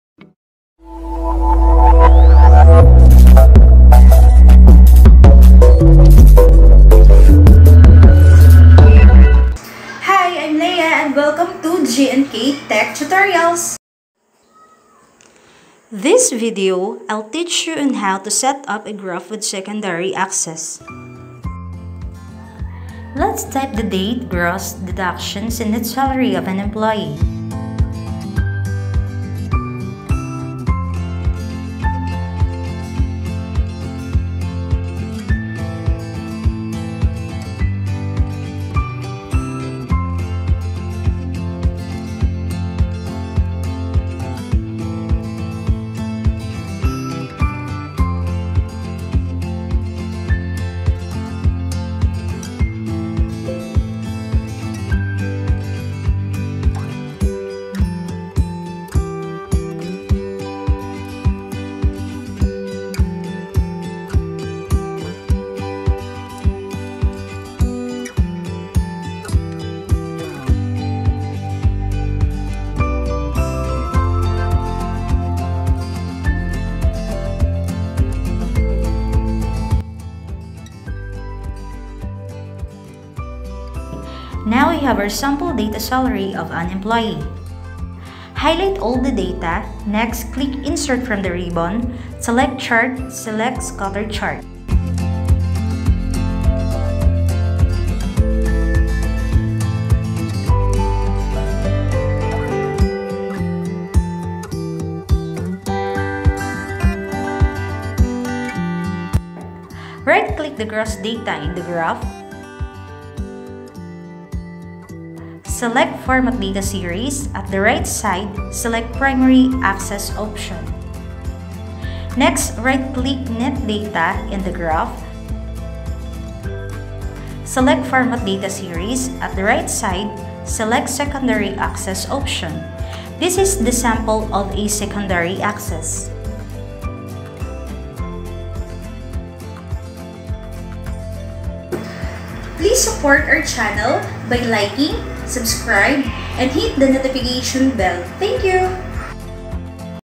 Easy way to Create a graph in excel with Secondary axis (all version)
In this video we cover how to create a graph with Secondary axis.

Key sections to this short tutorial video:
1. Create graph
2. Primary axis
3. Secondary axis